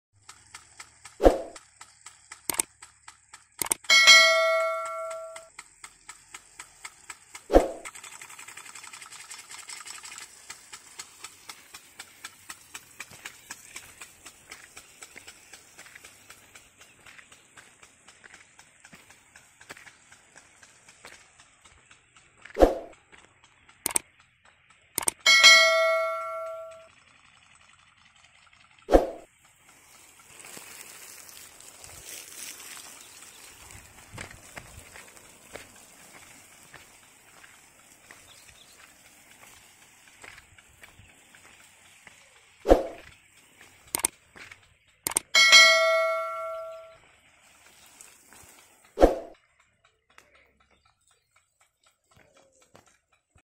Penguin HT - 40G | An Ultimate 1.25" Water Rain Gun | Effective Water Distribution | DVI
Penguin HT-40G - An ultimate 1.25" water rain gun by your trusted brand AUTOMAT. This water gun is highly engineered for effective water distribution in a variety of crops mainly sugarcane, tea, coffee, fodder crops. IT is fitted with a jet breaker screw to change the size of droplets and it comes with a range of 30 sqmtr. It comes with a full circle and part circle range for your convenience..